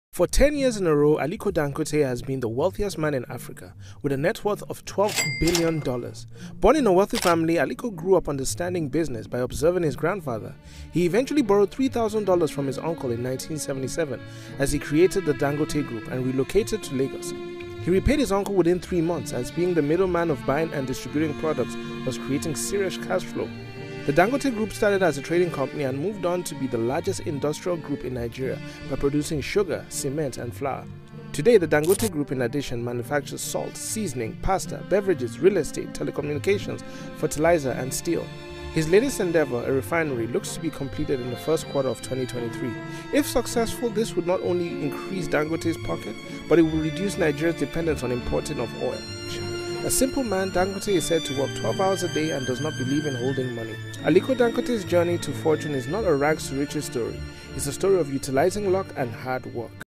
Aliko Dangote In 1 Minute
How much do you know about Aliko Dangote? The richest man in Africa?